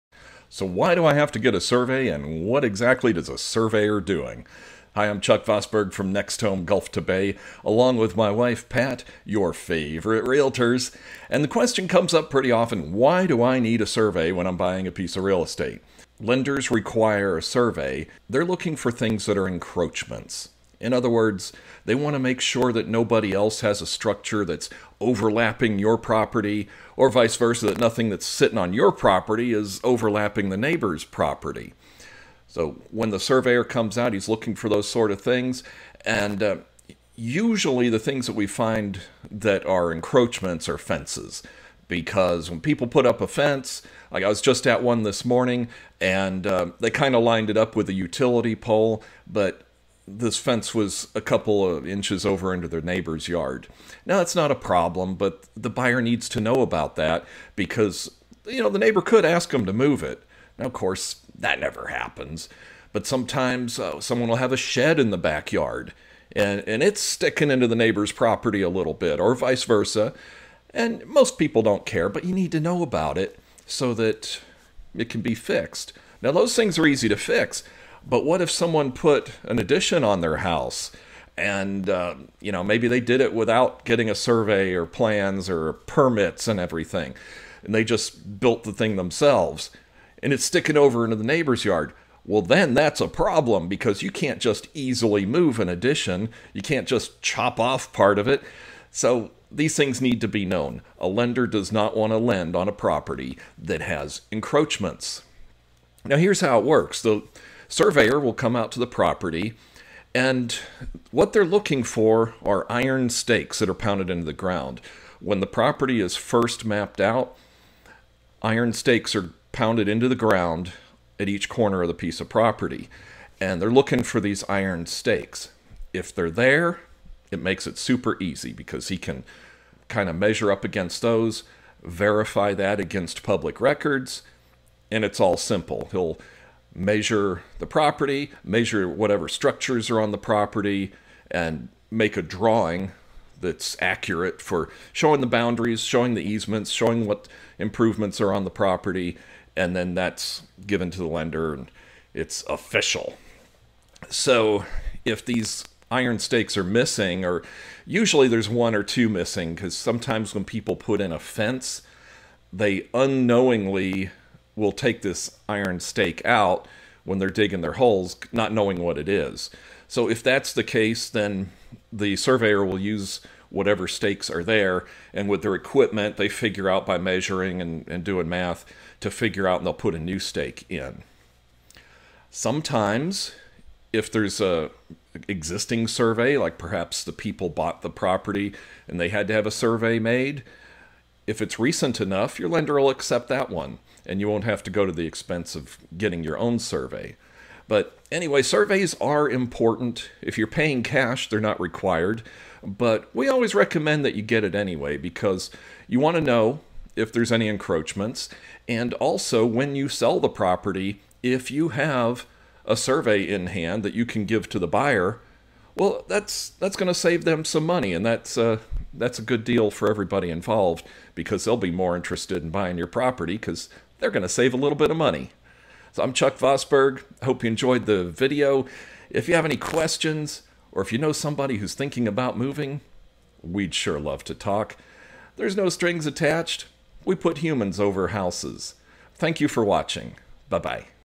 Why you need a survey before you close
There are a few good reasons lenders require a recent survey on your new home before closing. Here's why.